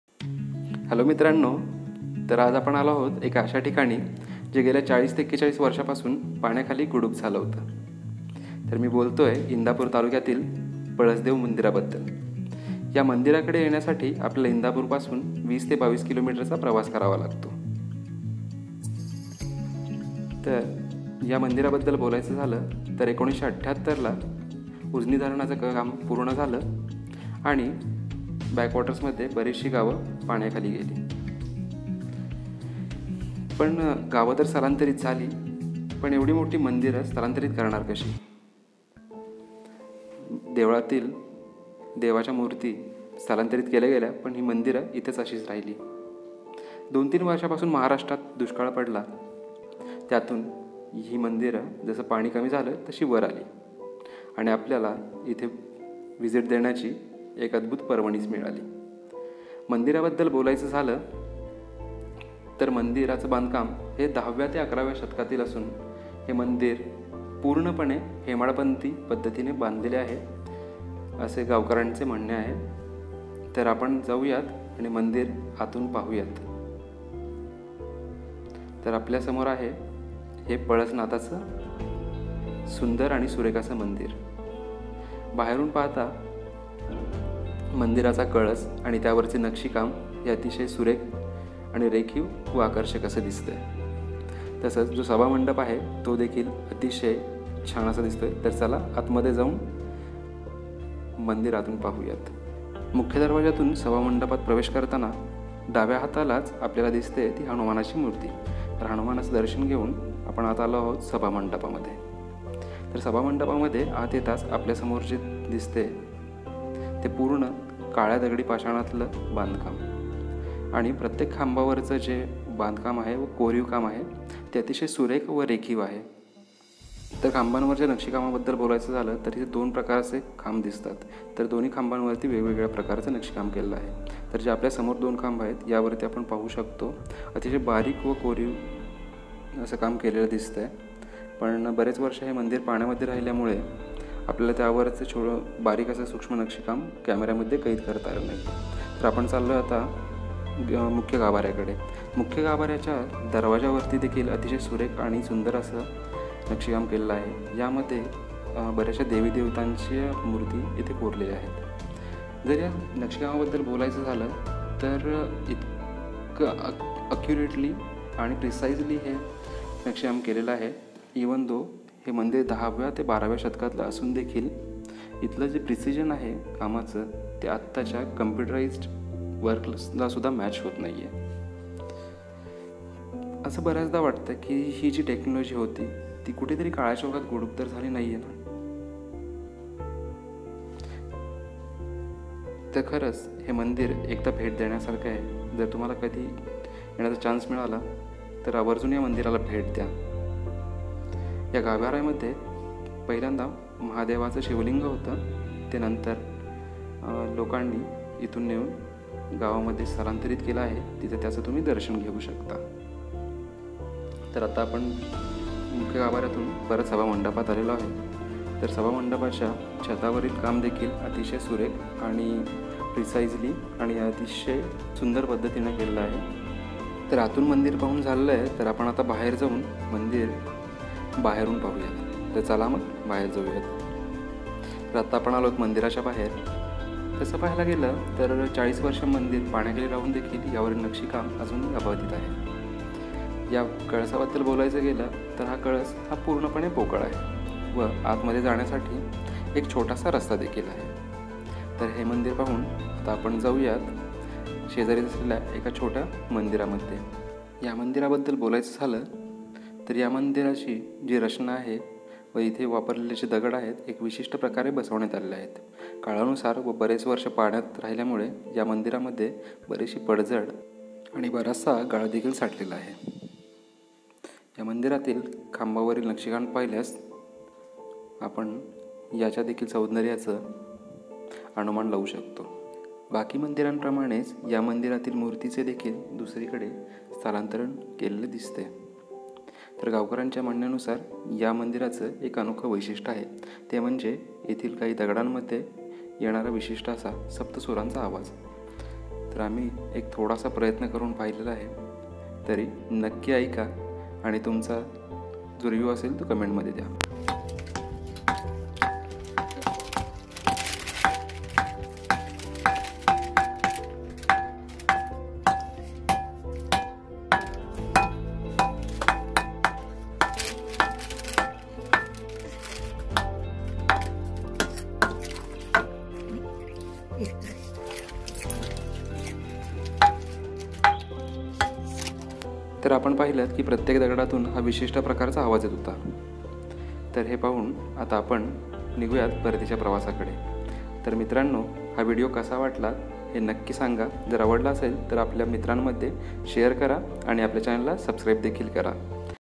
Palasdev Temple, Indapur 2019 । पळसदेव मंदीर, इंदापूर
या व्हिडिओ मध्ये आपण भेट देणार आहोत एका अशा मंदिराला जे गेल्या ४० वर्षापासून पाण्याखाली गुडूप झाल होत. तर हे नक्की कोणतं ठिकाण आहे हे पाहण्यासाठी व्हिडिओ पूर्ण पहा आणि यातील प्रमुख वैशिष्ट्य म्हणजे सप्तसुरांचा आवाज येणारे  मंदिरातील दगड  तर व्हिडिओ पूर्ण पहा आणि आवडल्यास share  करा, Like करा आणि आपल्या चॅनेलला subscribe  नक्की करा . .